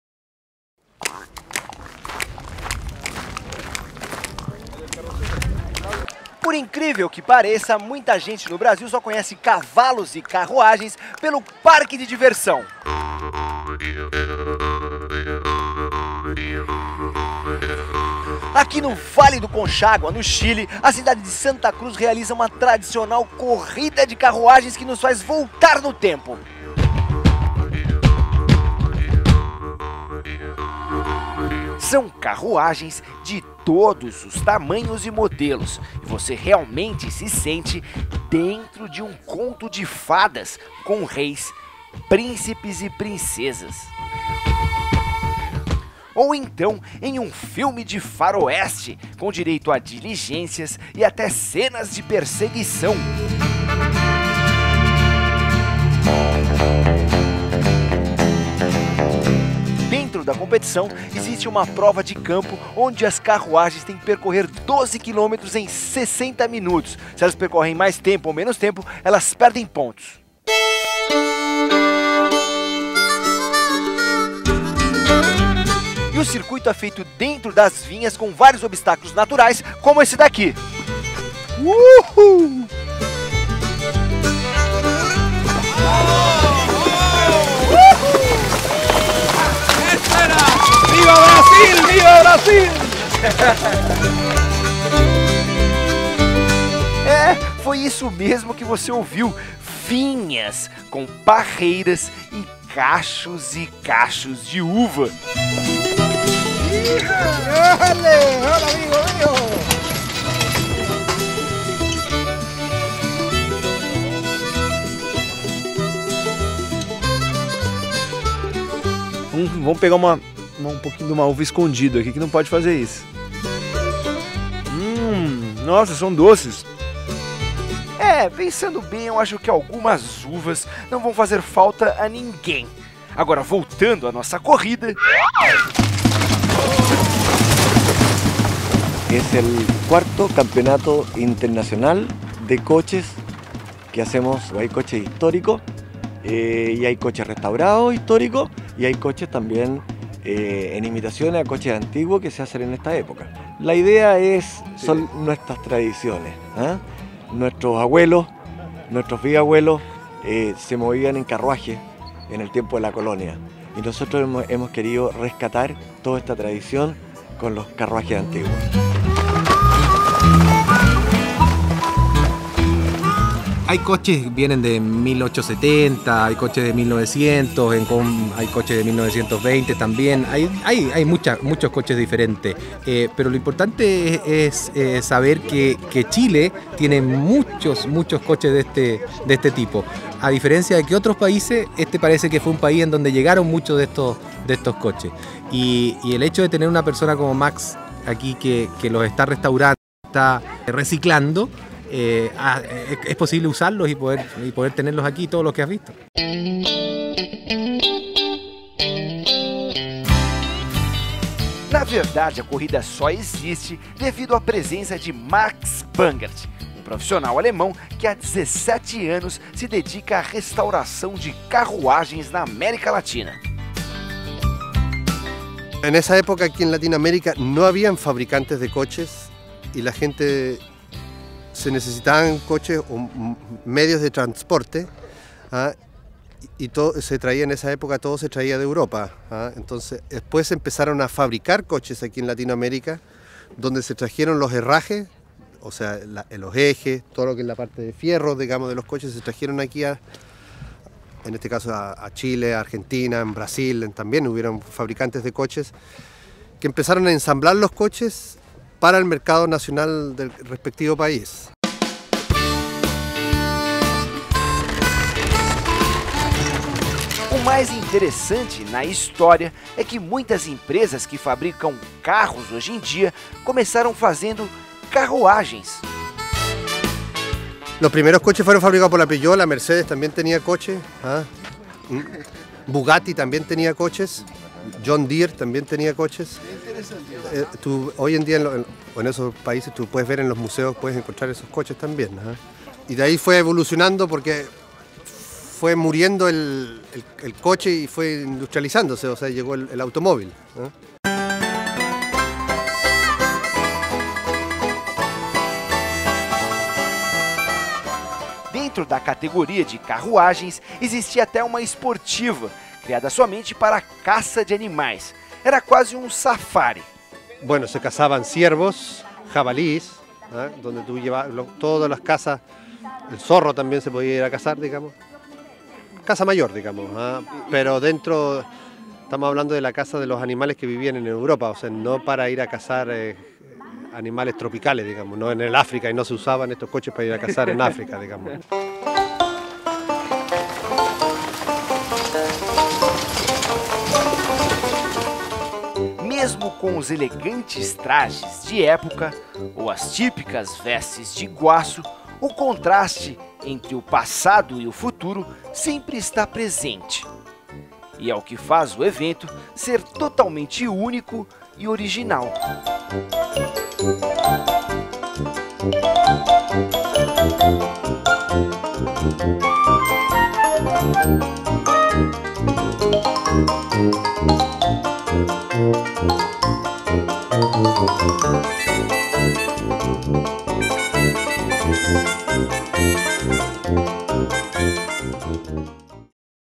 CORRIDA DE CARRUAGEM NO CHILE
Na cidade de Santa Cruz cituada no Vale do Colchagua no Chile  todos os anos existe uma tradicional corrida de Carruagens Antigas .  A região é famosa por ser produtora dos melhores  tintos chilenos e a corrida acontece em meio as vinhas carregadas de uvas .